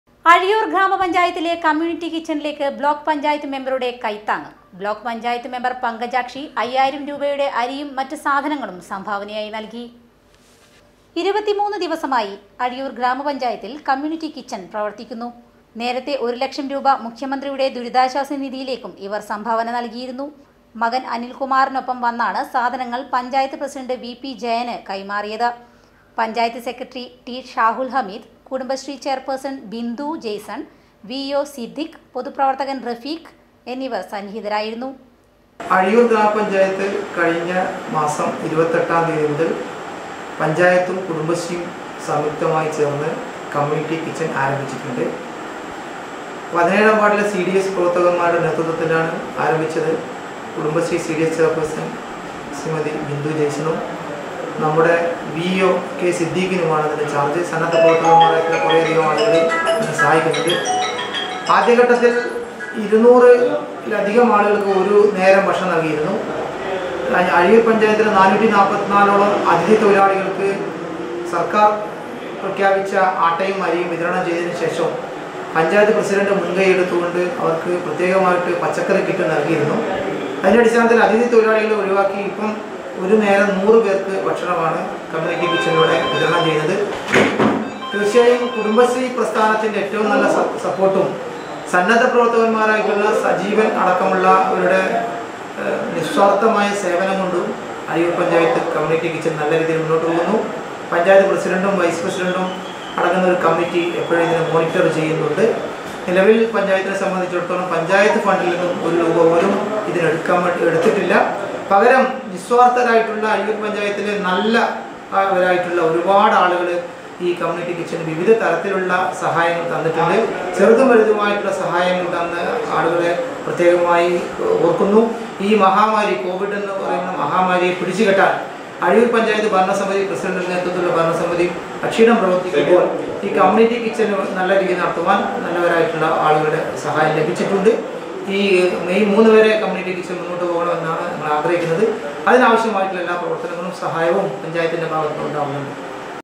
COMMUNITY KITCHEN AZHIYUR GRAMAPANJAYTH
അഴിയൂർ ഗ്രാമപ്പഞ്ചായത് കമ്മ്യൂണിറ്റി കിച്ചണിലേക്കു ബ്ലോക്ക്പഞ്ചായത് മെമ്പറുടെ സഹായം